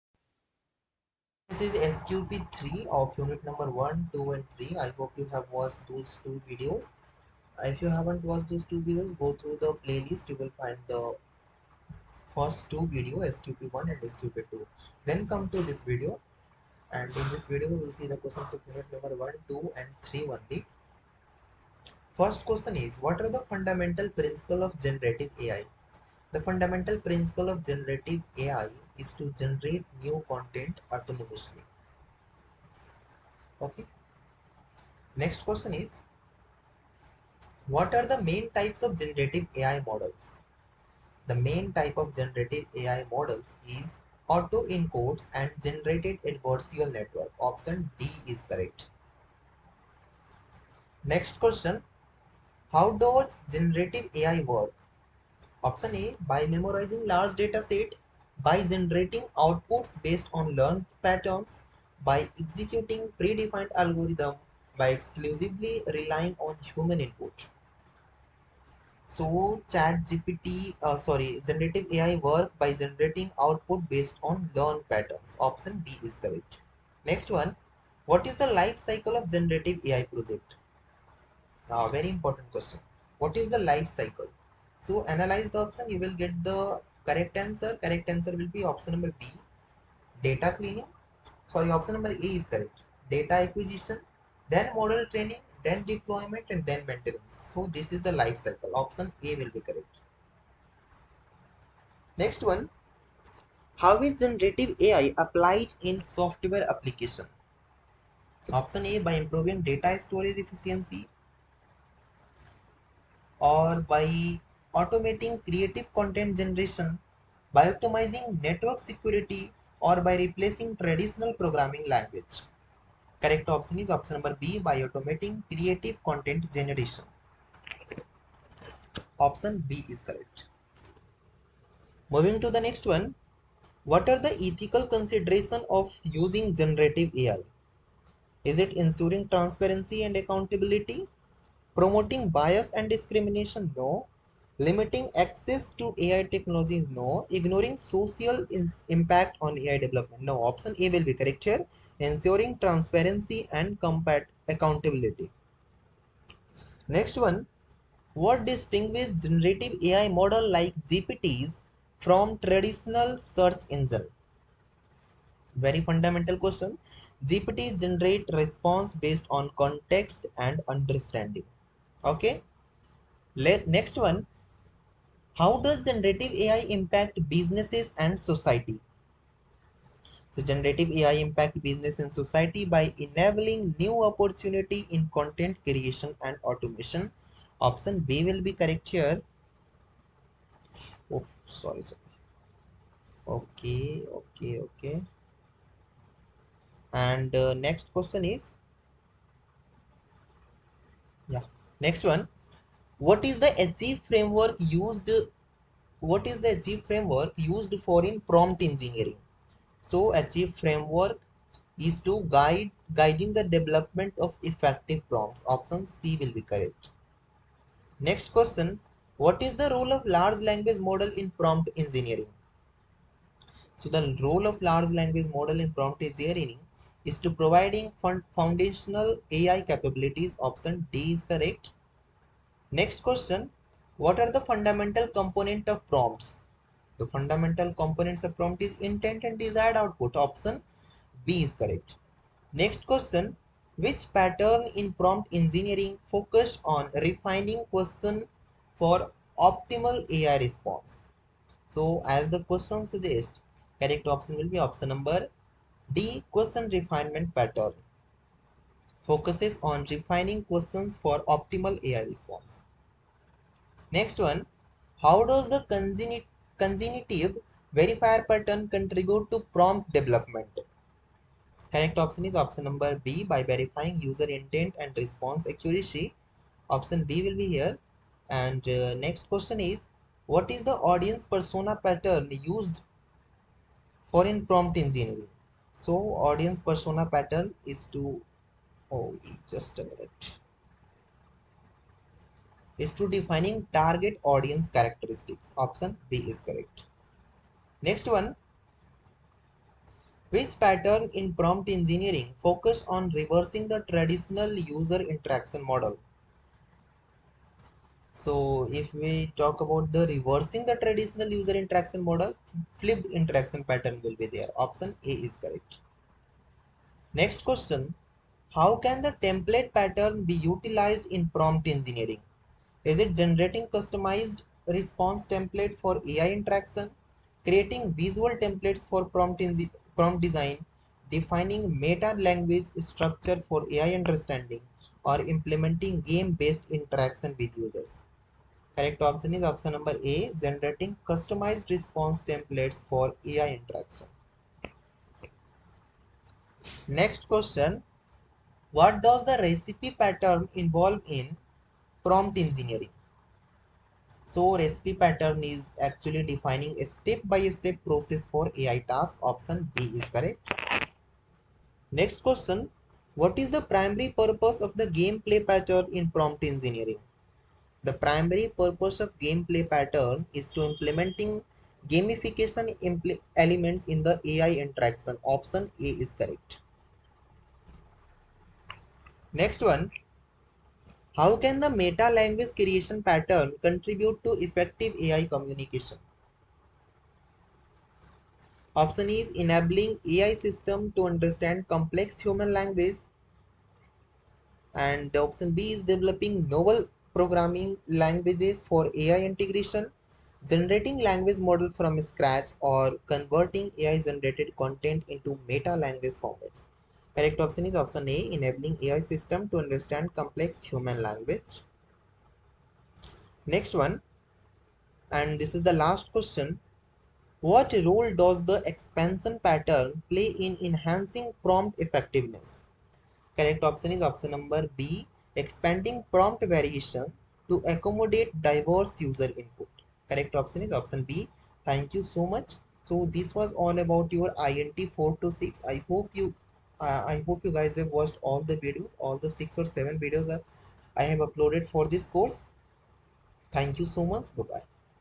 Sample Question Paper (Part 3) | MCQs | Generative AI | INT426 | LPU
In this video, I will let you know Most important and Frequently asked MCQs of Unit 1, 2 and 3 of INT426, : GENERATIVE ARTIFICIAL INTELLIGENCE which is Unit 1
Introduction to Generative AI: Fundamentals of Generative AI, Generative AI
model types, Applications of Gen AI, How Gen AI works, Lifecycle of a Gen AI
project, Gen AI in software applications, Gen AI in Business and Society,
Difference between GPTs and search engines, Ethical and responsible AI,
Unit 2
Prompt Engineering: Transforming computing, The ACHIEVE framework,
Introduction to Large Language Models, fundamentals of prompt, prompt patterns,
prompt tuning
Prompt Pattern I: question refinement pattern, cognitive verifier pattern, audience
persona pattern, flipped interaction pattern
Unit 3
Prompt Pattern II: Game Play Pattern, Template Pattern, Meta Language
Creation Pattern, Recipe Pattern, Alternate approaches pattern
Prompt Pattern III: Combining Patterns, Expansion patterns, Menu Action
Patterns, Check List Pattern, Tail Generation Pattern, Semantic Filter Pattern

SQP Part 2 MCQs :-

Topics covered in this video:
Most important MCQs of INT426